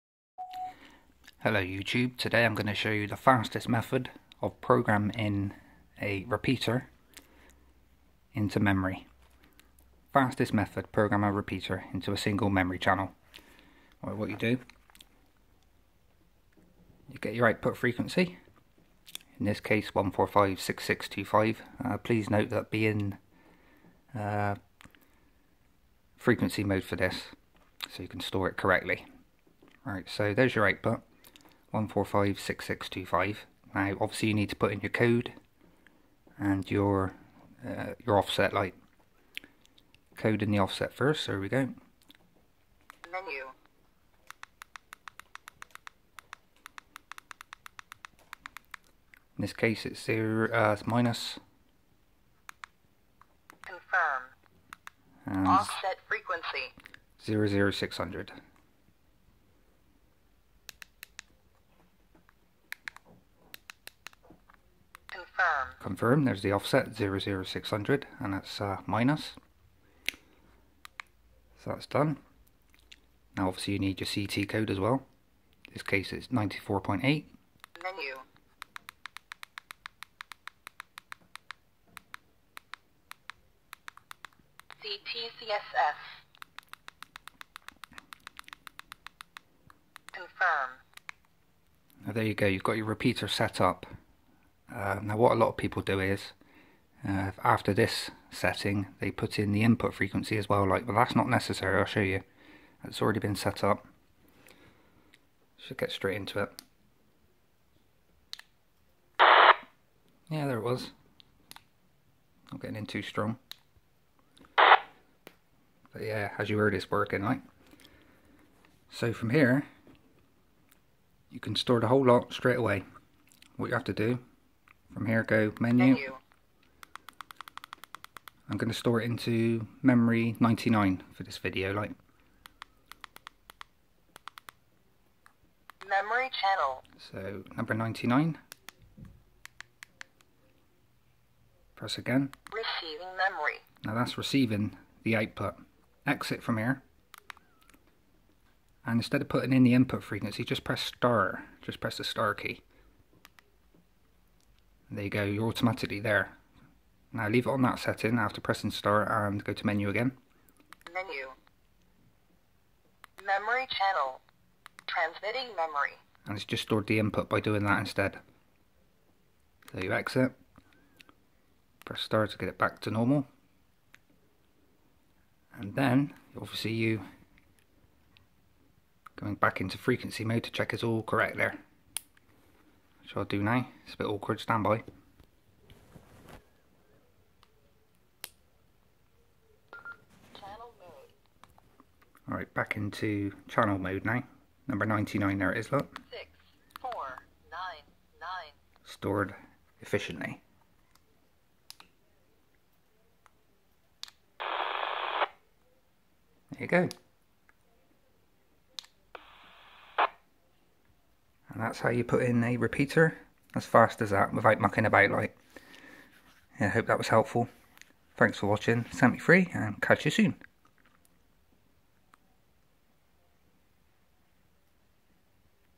Baofeng (fastest way to programme a repeater!)
A video showing the fastest method of manually programming a repeater into memory, without the need of entering the input frequency.